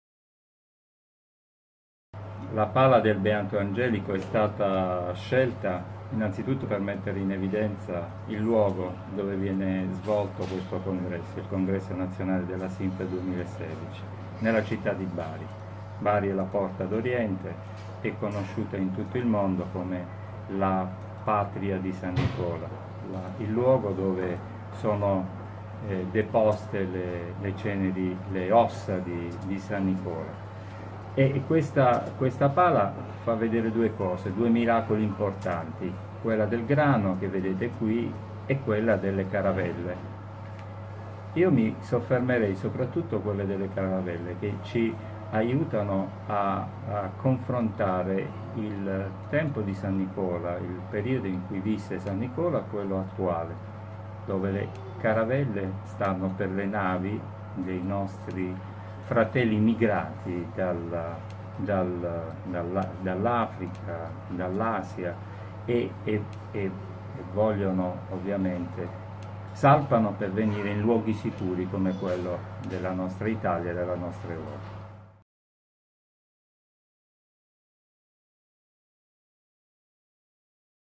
L’immagine del 44mo congresso Simfer: i miracoli di san Nicola
È “San Nicola salva la nave” del Beato Angelico, facente parte della Pala di Perugia (1438) l’emblema del 44mo congresso nazionale della SImfer, la Società italiana Medicina fisica e riabilitativa, che si terrà a Bari dal 23 al 26 ottobre 2016.
Il riquadro, custodito nella Pinacoteca Vaticana, è il secondo di un trittico che forma la predella della pala. È occupato quasi interamente dalla scena delle navi granarie. San Nicola approfitta della sosta ad Andriake di alcune navi che trasportano grano da Alessandria d'Egitto a Costantinopoli, per farsi dare un po' di grano per i miresi (all'arrivo delle navi a Costantinopoli, non verrà riscontrato alcun ammanco). Sulla destra, in una nave, alcuni naviganti invocano il santo che li salva dalla tempesta.
In questo video Pietro Fiore, presidente del 44mo congresso, spiega le ragioni della scelta di un’immagine di sei secoli fa e che tuttavia contiene motivi di grande attualità. Oltre naturalmente a riportarci alla città sede del congresso, di cui San Nicola è patrono.